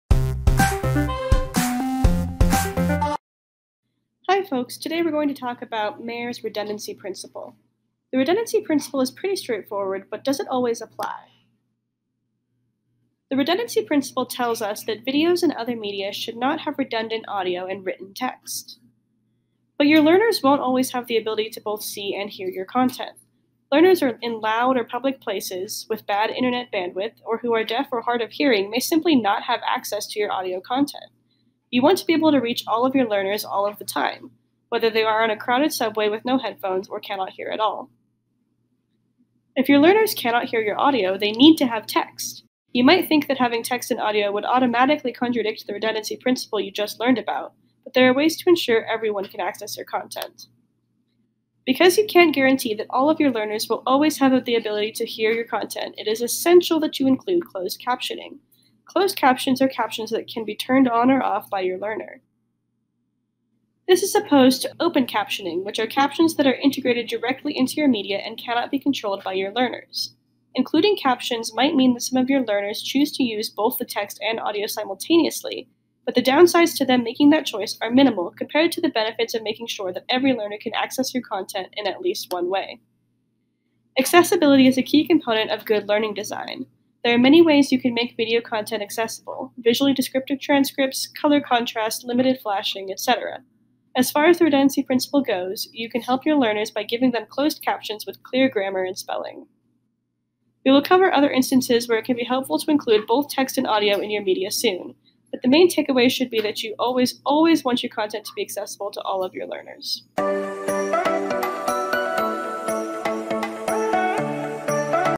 Mayer's Redundancy Principle Explained
Dr. Richard Mayer's Redundancy Principle states that "People do not learn better when printed text is added to graphics and narration. People learn better from graphics and narration than from graphics, narration, and printed text, when the lesson is fast paced." In this elearningdesigners.org video scripted, produced, and narrated by Courtney Stephens, you'll learn when the redundancy applies and conditions when it doesn't.